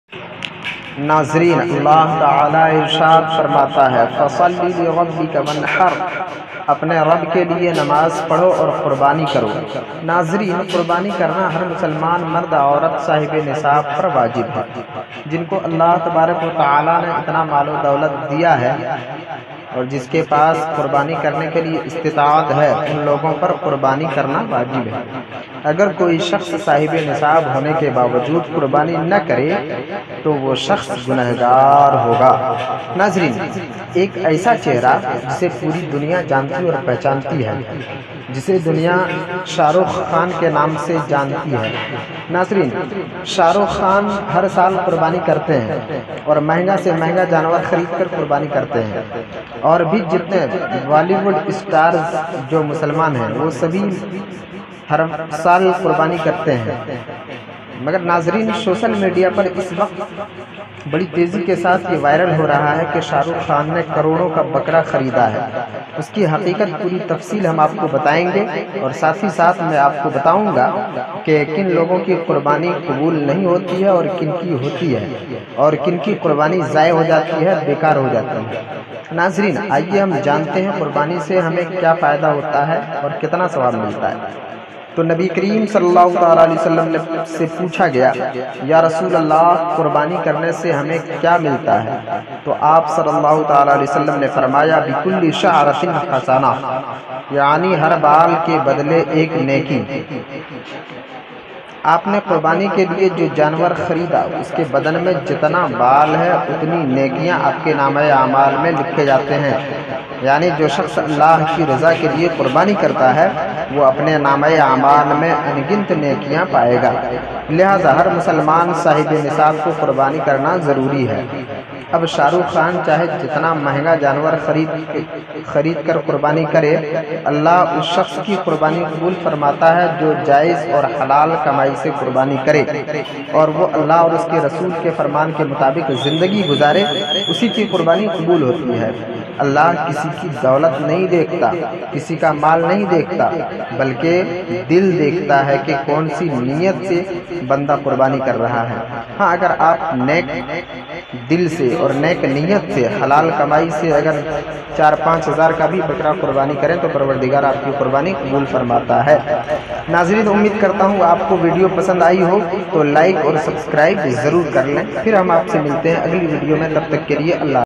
Shahrukh Khan ne kharida Bakra Eid 2020 ka bakra
शाहरुख खान ने 7 करोड़ का बकरा खरीदा